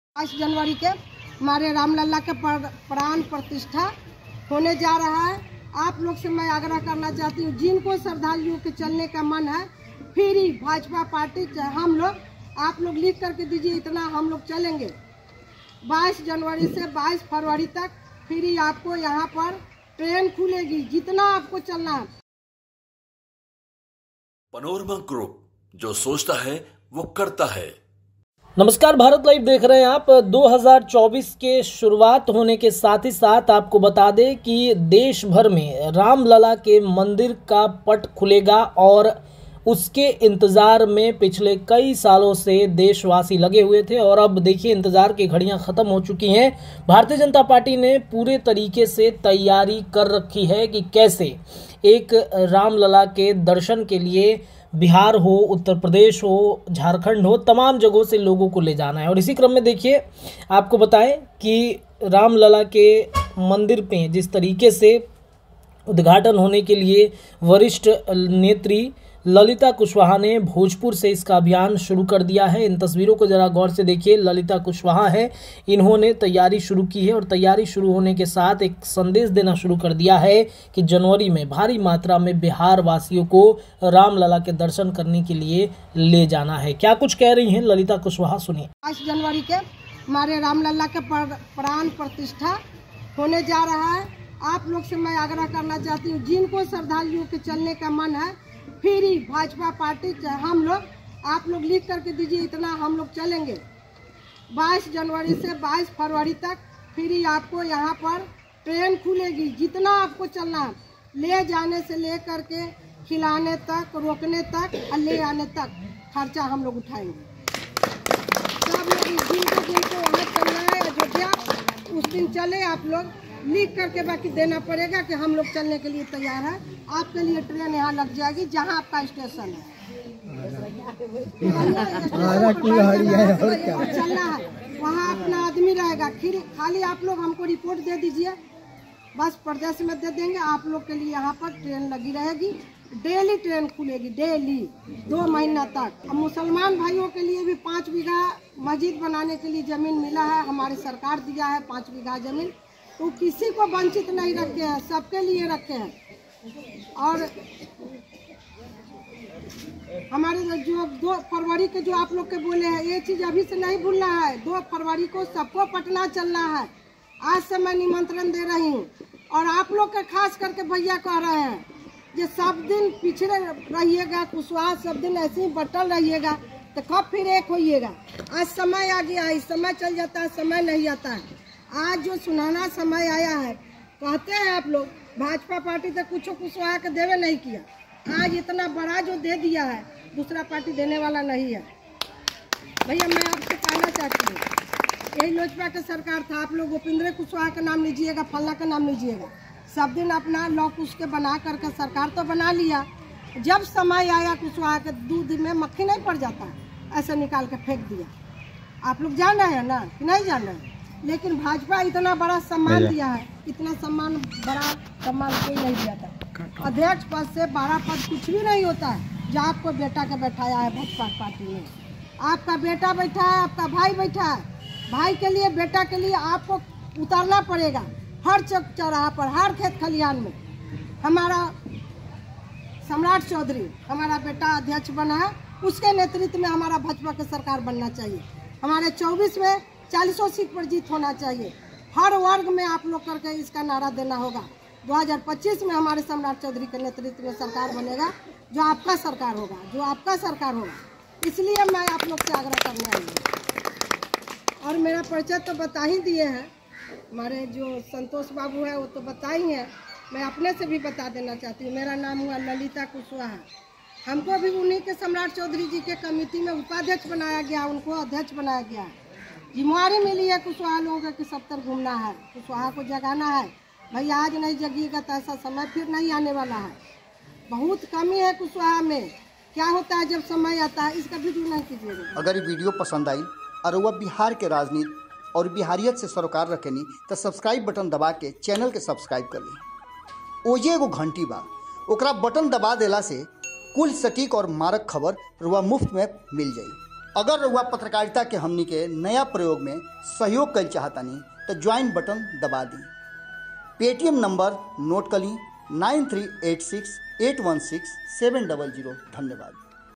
मिशन 2024 ! भाजपा का अयोध्या में राममंदिर उद्घाटन में शामिल होने के लिए वरिष्ठ नेत्री ललिता...
मिशन 2024 ! भाजपा का अयोध्या में राममंदिर उद्घाटन में शामिल होने के लिए वरिष्ठ नेत्री ललिता कुशवाहा ने भोजपुर में शुरू किया अभियान !